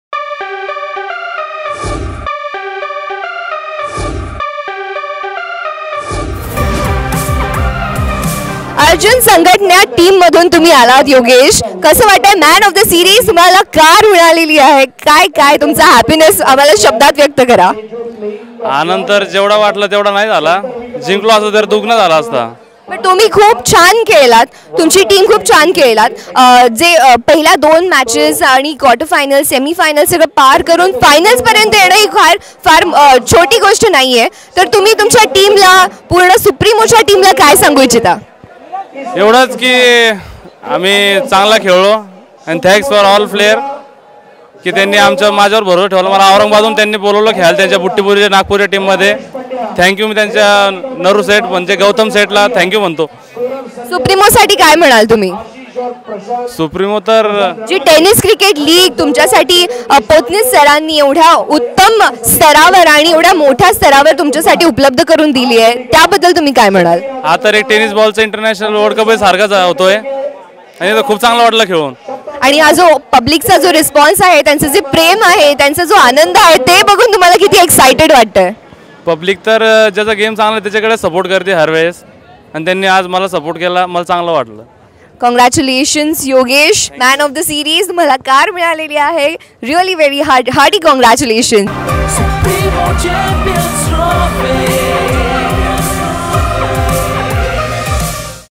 Man of The Series of Supremo Trophy 2017 Mr Yogesh Anna of Arjun Sanghatana Team
Man of The Series of Supremo Trophy 2017 Mr Yogesh Anna of Arjun Sanghatana Team, wins the alto car